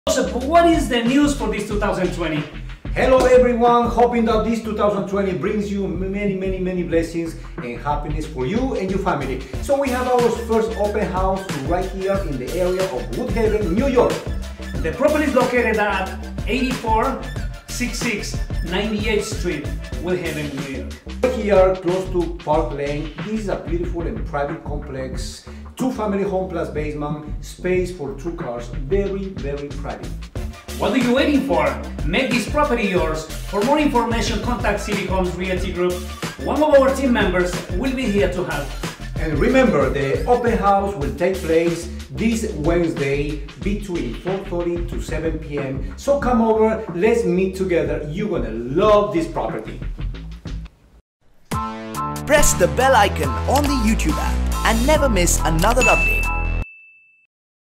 YOUR INVITED TO OUR OPEN HOUSE :)
City Homes Realty cordially invites everyone to attend our open-house this Wednesday January 8, 2019 from 4:30-7pm. Start 2020 off right with this great home at a great deal. Your family will fall in love instantly!

84-66 98 St, Woodhaven NY
Open House from 4:30-7pm Wednesday January 8, 2020 :)